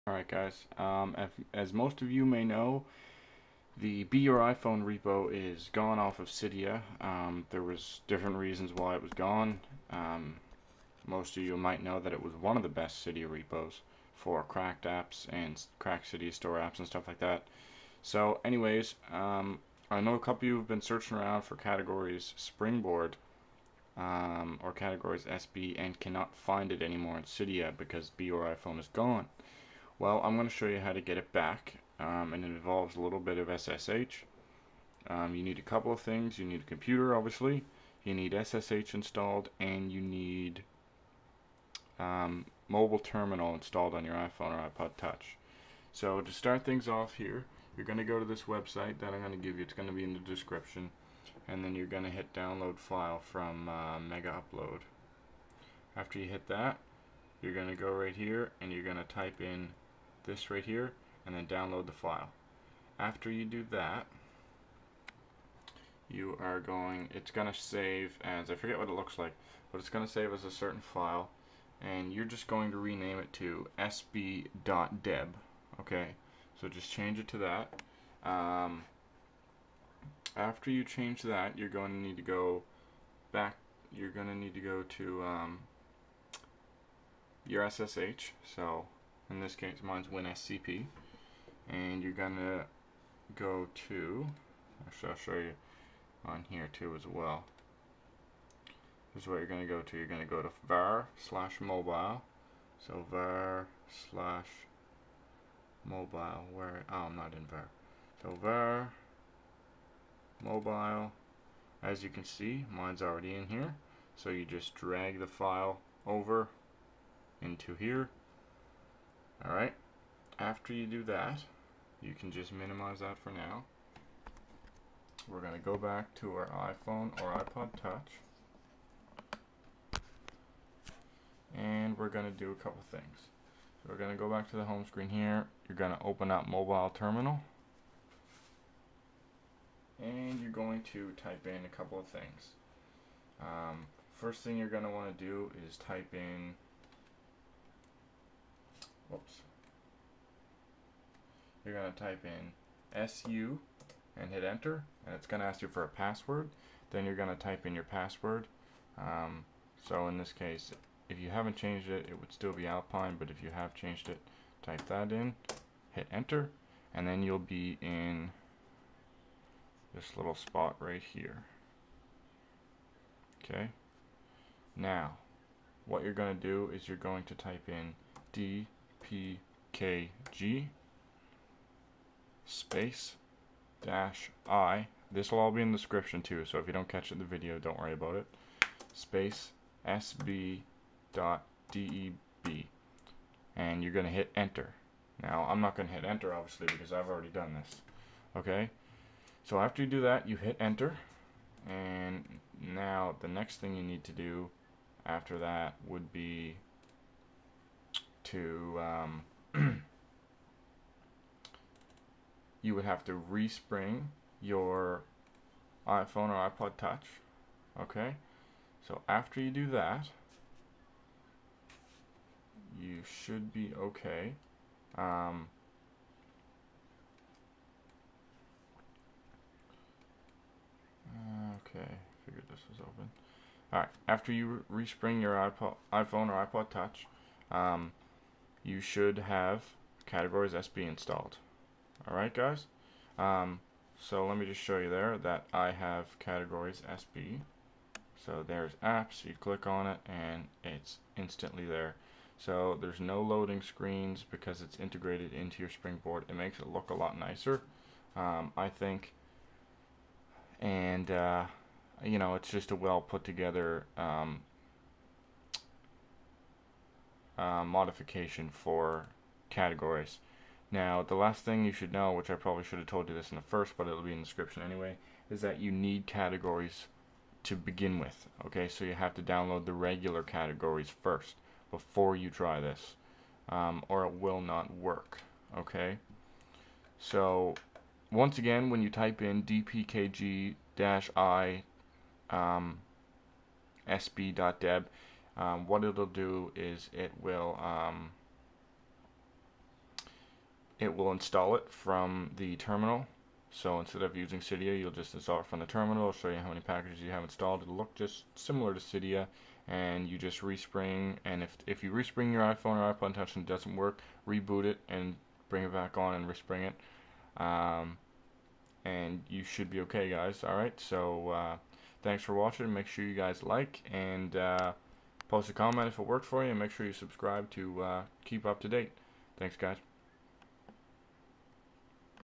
How to get CategoriesSB free for iPhone, now that BYIP repo is gone!!
Heres how to get CategoriesSB Free for the iPhone or iPod Touch without using cydia at all!!

You must first have Categories installed (free from cydia), SSH, and mobile terminal.